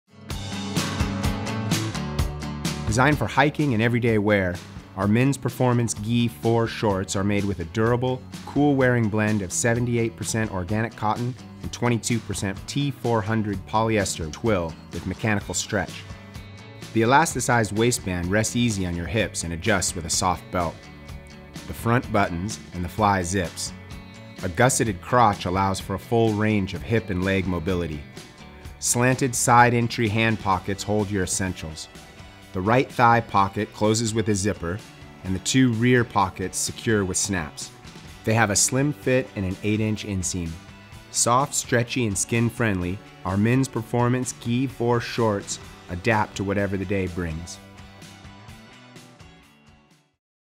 Patagonia Men's Performance Gi IV Shorts - 8"
Everyday trail-to-town hiking shorts made of a durable 78% organic cotton/22% polyester twill with mechanical stretch.